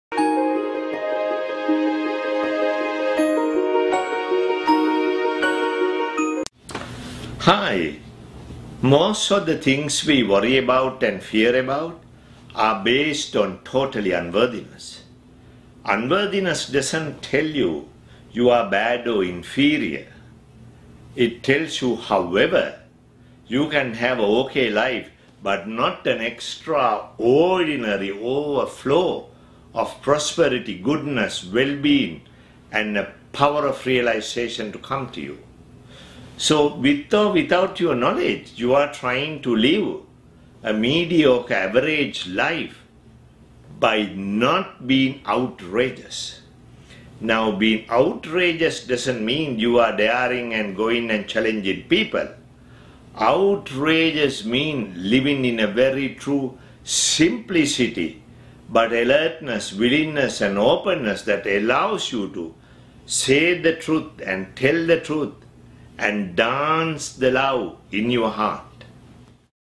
Live an Extraordinary Life | Flashes of Brilliance | Tilak Fernando
Tilak Fernando talks on different topics which helps you unleash your true potential and fascination for life in a series of short talks named "Flashes of Brilliance".